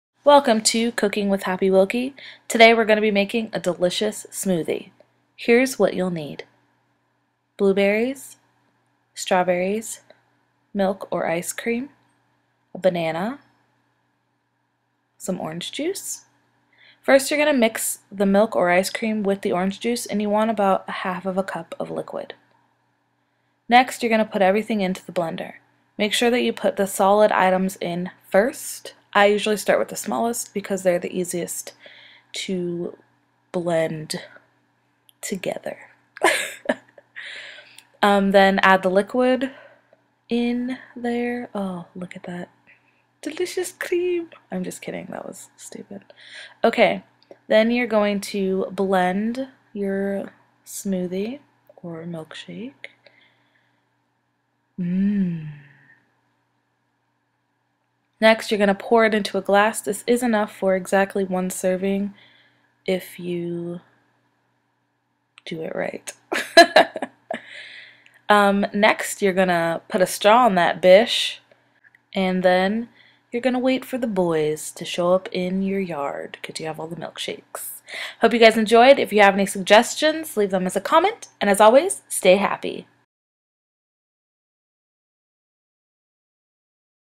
COOKING WITH WILKIE: FRUIT SMOOTHIE
Sometimes all you need is a good fruit smoothie! Try this one out and tell me what you think!